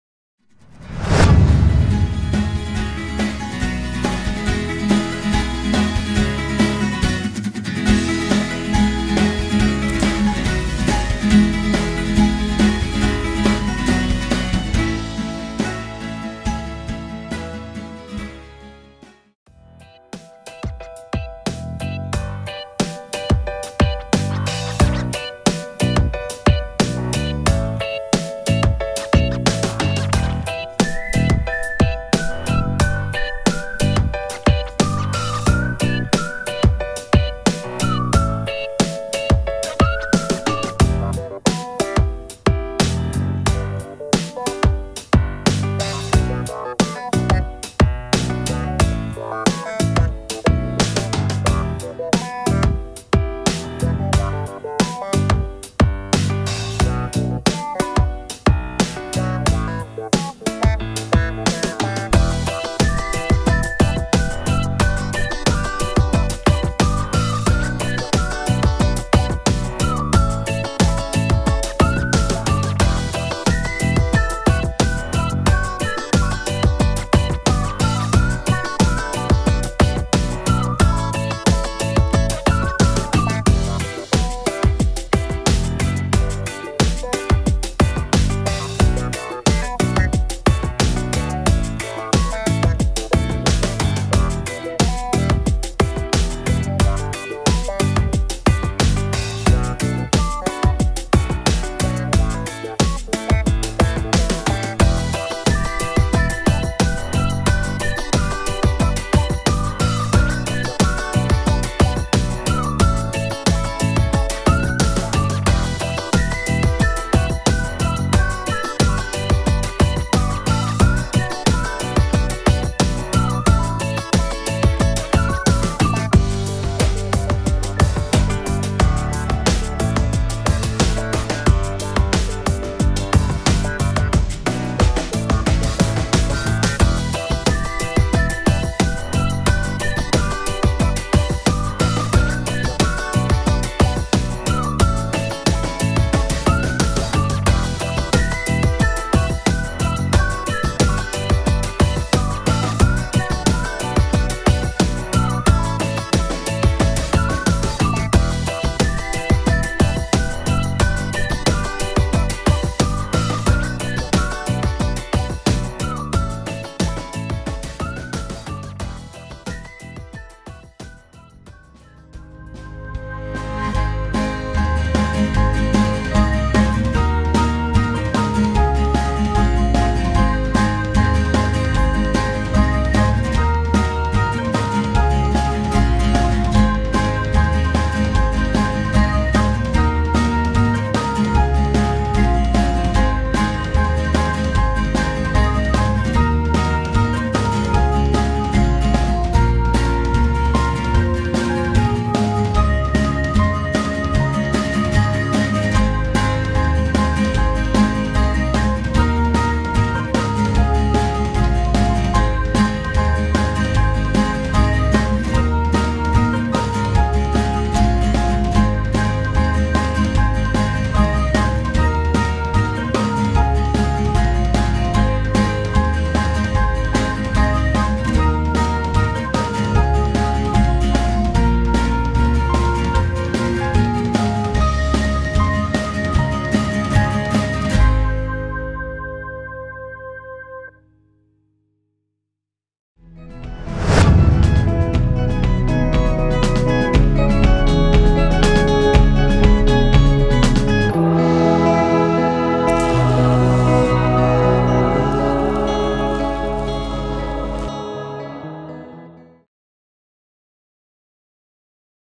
Minecraft Puzzle | Pre-K Art
Make your own Minecraft puzzle.
You can glue it after playing with it. You can use different kinds of color tools to color it (color pencils, crayons, markers, paints, etc.).